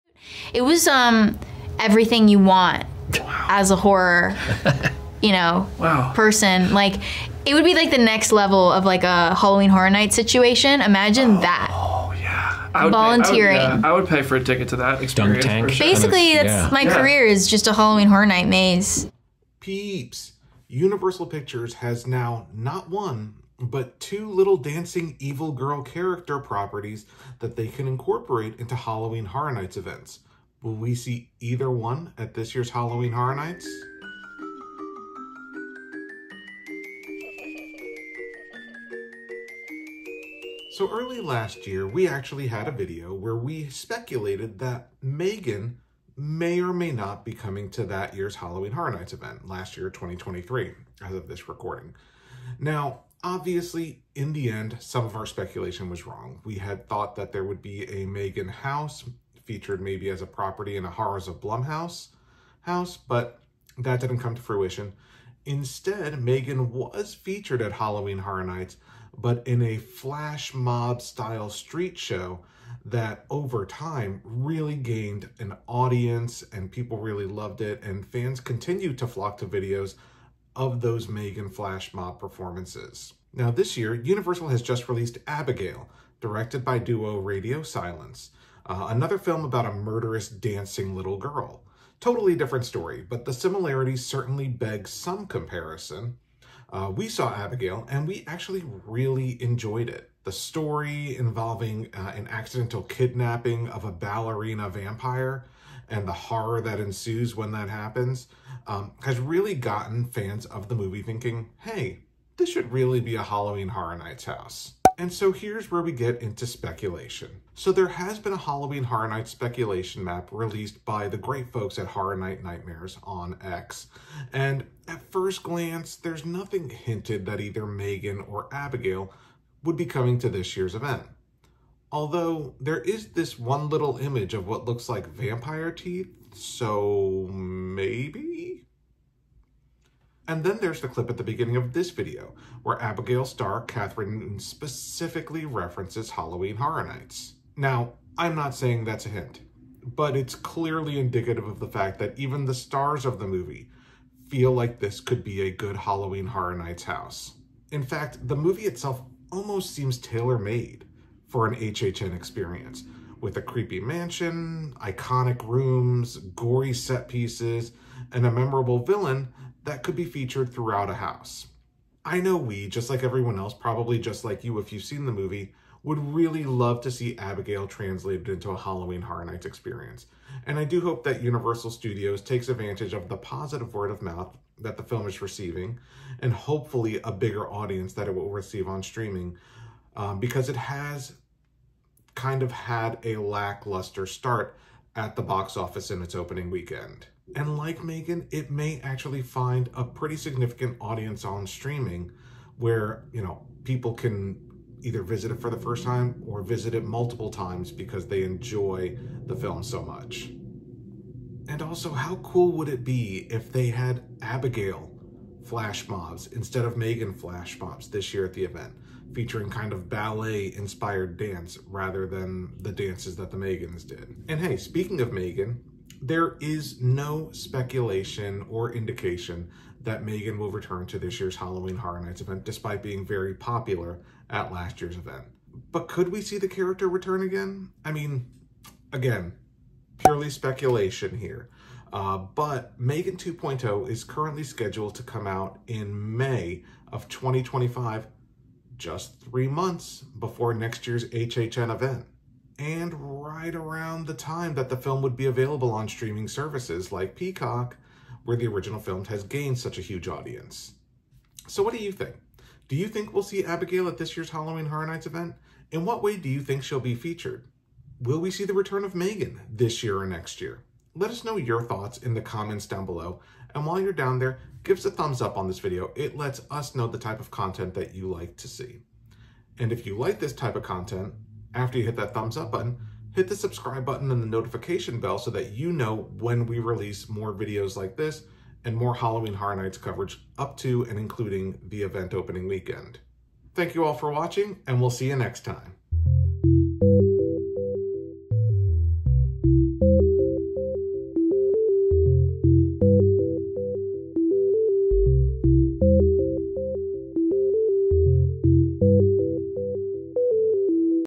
Could MEGAN or ABIGAIL come to HALLOWEEN HORROR NIGHTS 2024? HHN33 Speculation
Universal Pictures now has not one, but TWO little evil girl dancing characters to incorporate into future HHN events - will we see either character this year at HHN?